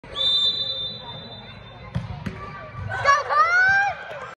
1 Futsol Penalty Kick - Kaan #1
Play off final 4 overtime penalty kicks

Kaan kicked first